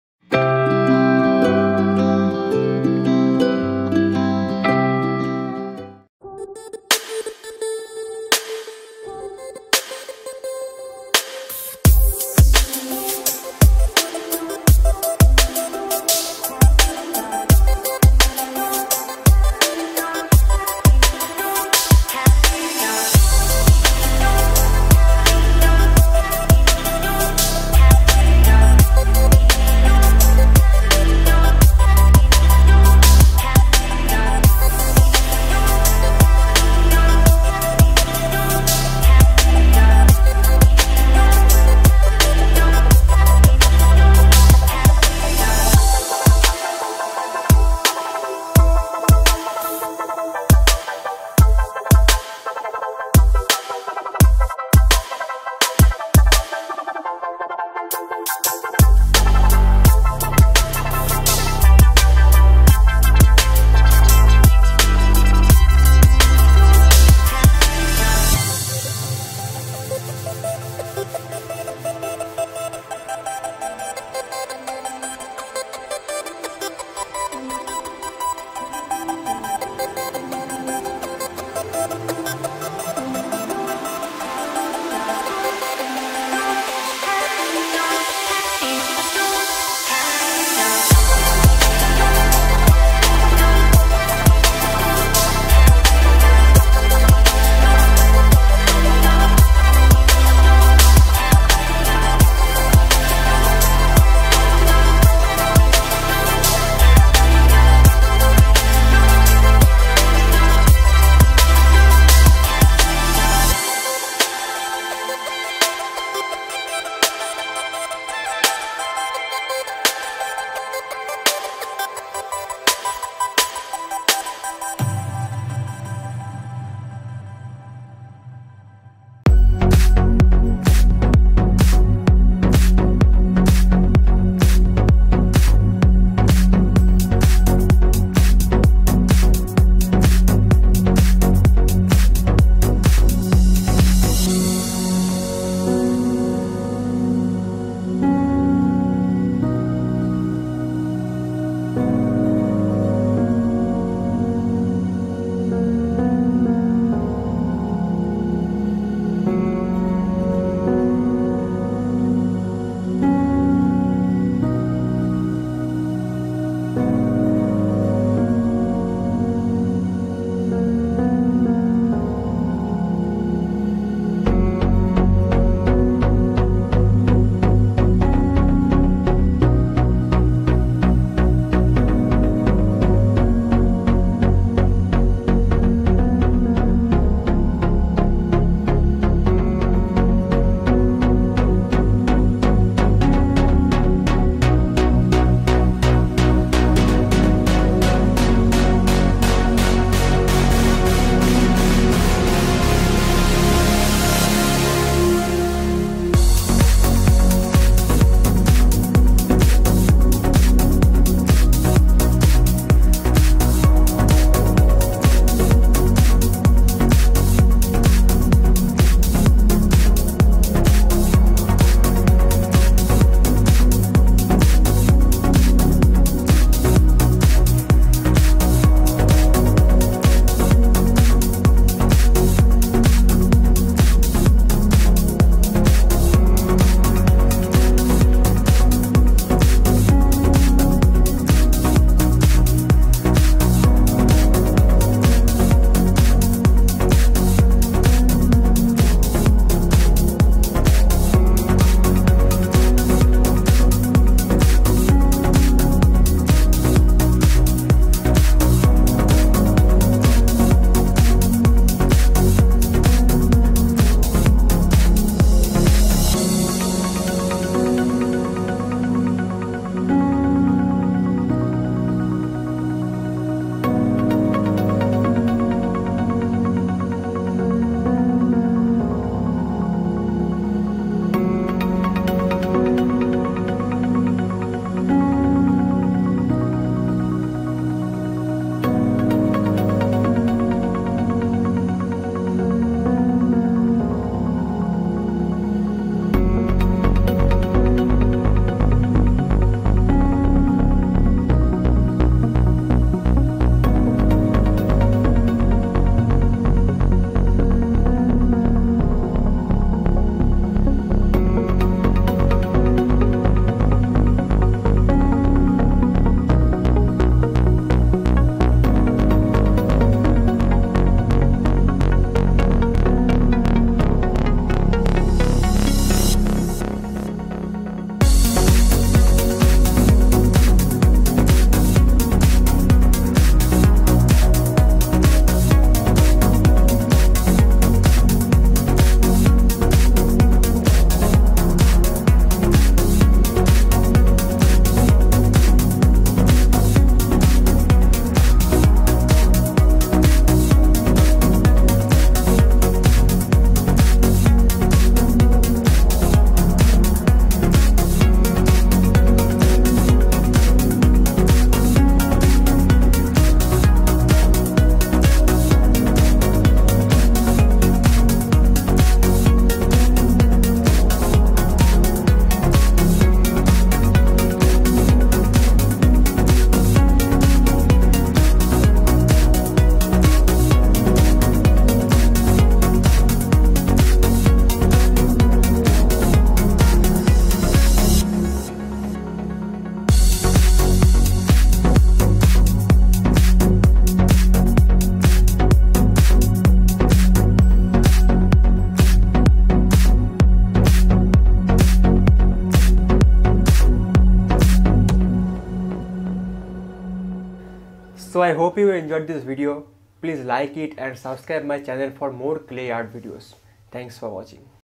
Horse making with clay | clay horse modelling | miniature clay crafts making
I made this miniature clay crafts is very easy and simply. I hope it's very helpful for biggeners.